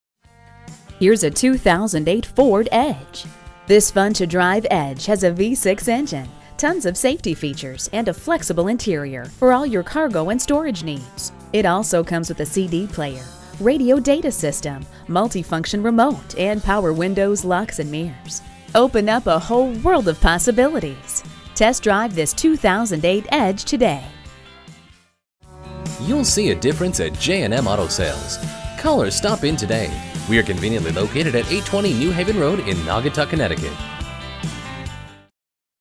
2008 Ford Edge Awd Naugatuck CT Hartford, CT #088473 - SOLD
Year: 2008
Make: Ford
Model: Edge Awd
Trim: 4d Wagon SEL
Engine: -1 Cylinders
Color: Grey
Mileage: 89510
Stock #: 088473
VIN: 2FMDK48C48BA98473
An Impressive, All Wheel Drive Edge SEL with an Accident-Free History Report and an Included Warranty!

Address:
820 New Haven Rd.